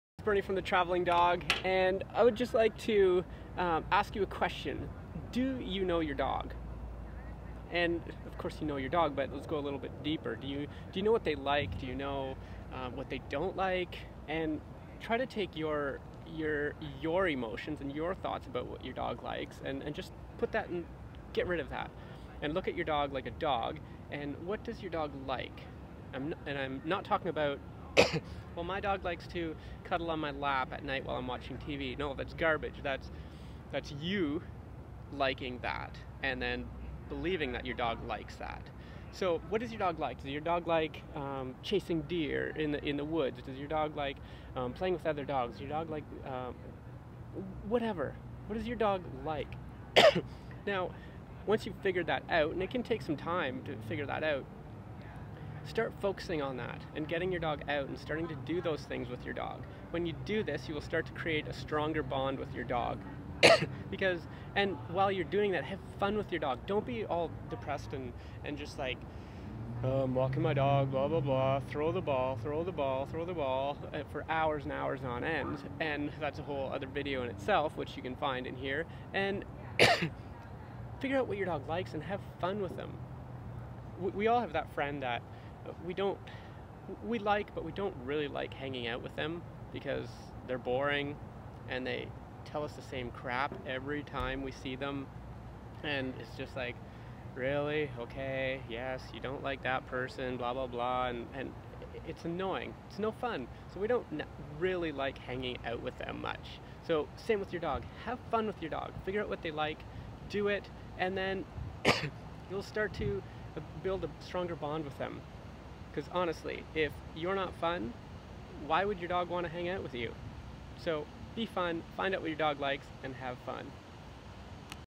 Do You Know Your Dog
Do You Know Your Dog? I have found that most people don't really know their dog. Sometimes we think we do but its often deeper then we think.